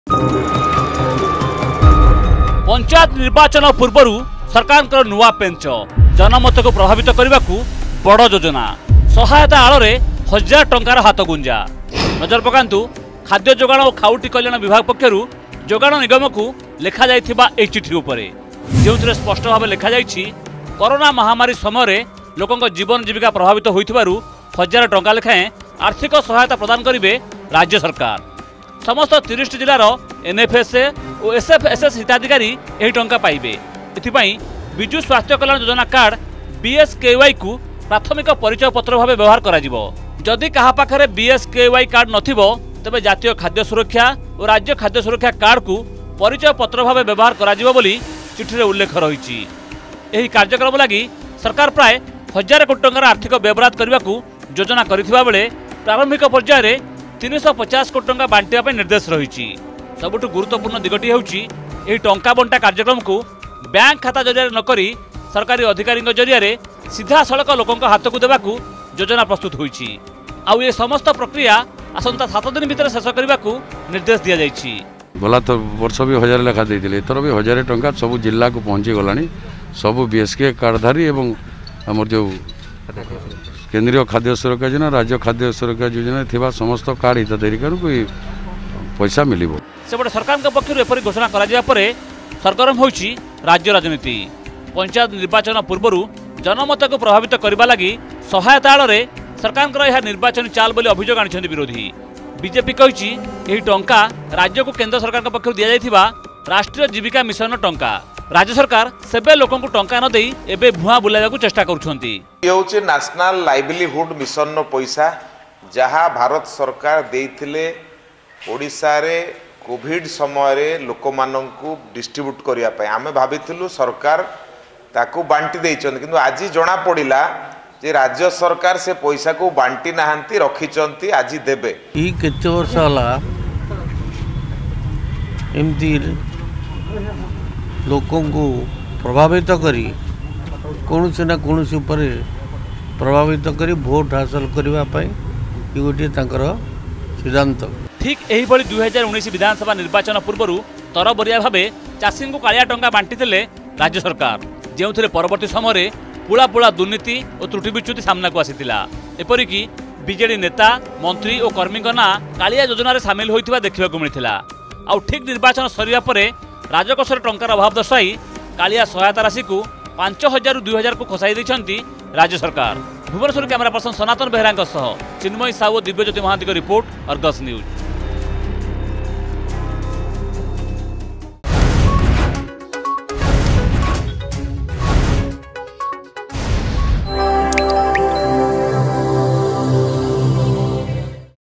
ପଞ୍ଚାୟତ ନିର୍ବାଚନ ପୂର୍ବରୁ ସରକାରଙ୍କ ନୂଆ ପେଞ୍ଚ
ଜନମତକୁ ପ୍ରଭାବିତ କରିବାକୁ ବଡ ଯୋଜନା  I ସହାୟତା ଆଳରେ ହଜାରେ ଟଙ୍କାର ହାତଗୁଞ୍ଜା ! ନଜର ପକାନ୍ତୁ ଖାଦ୍ୟ ଯୋଗାଣ ଓ ଖାଉଟି କଲ୍ୟାଣ ବିଭାଗ ପକ୍ଷରୁ ଯୋଗାଣ ନିଗମକୁ ଲେଖା ଯାଇଥିବା ଏହି ଚିଠି ଉପରେ I